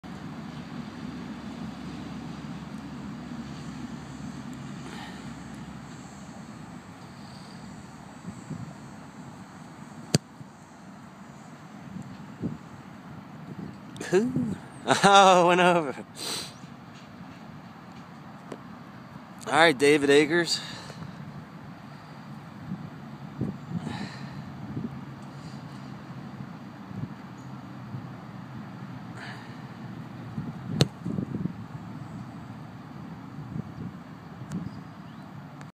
Taylor Russolino 2/2 57 yards
57 yard field goal, slight breeze at back